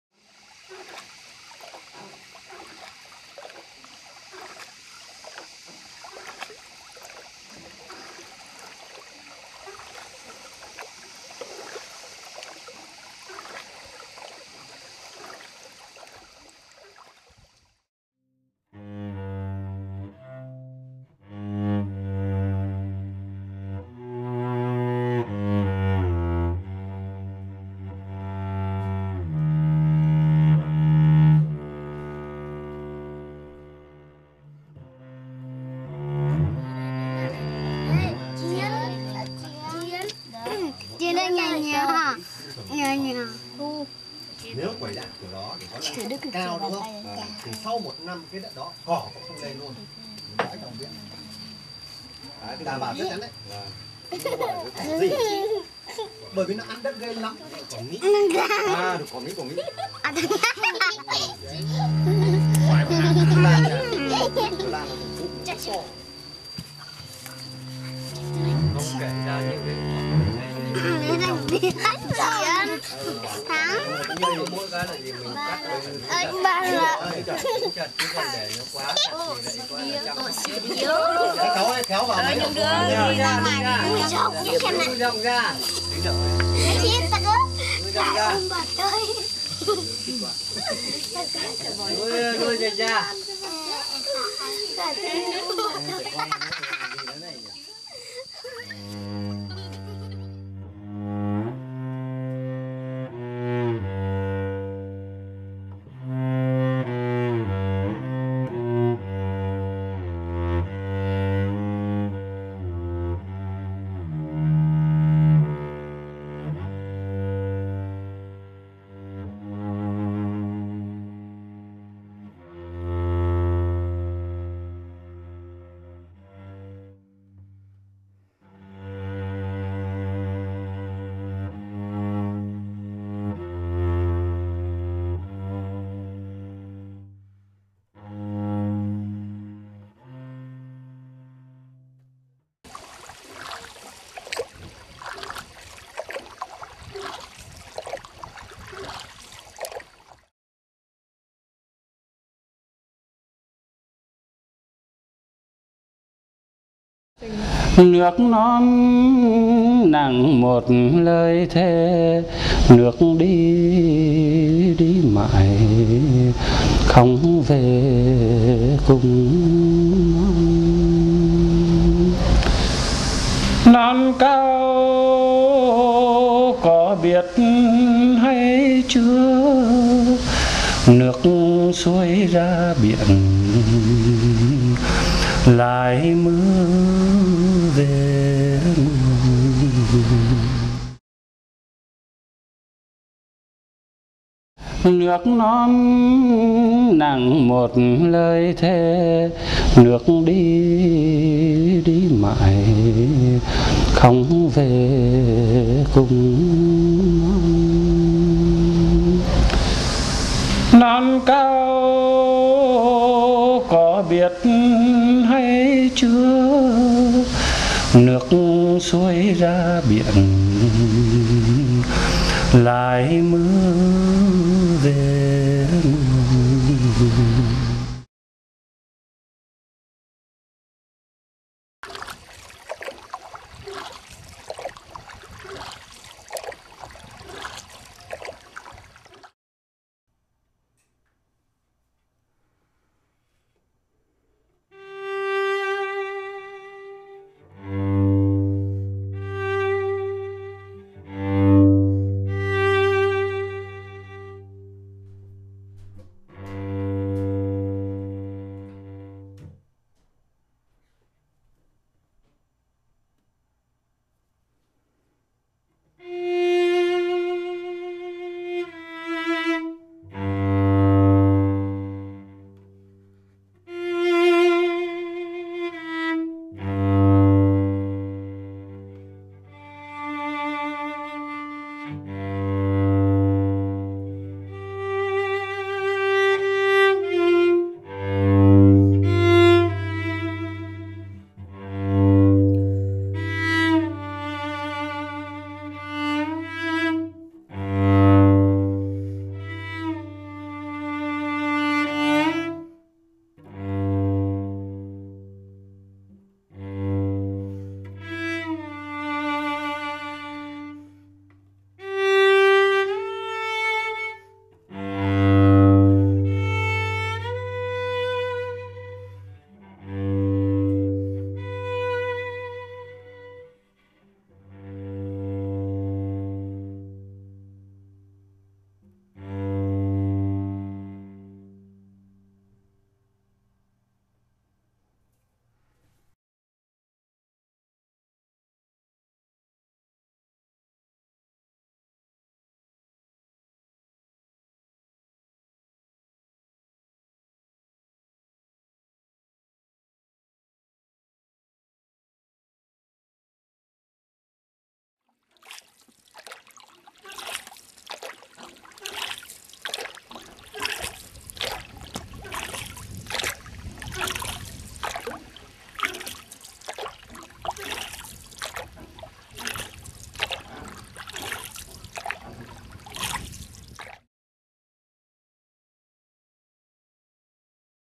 Vietnam: 3 short poems
Three takes on Vietnamese culture and society today.

Filmed and edited by Isabelle Carbonell.
Izaca Productions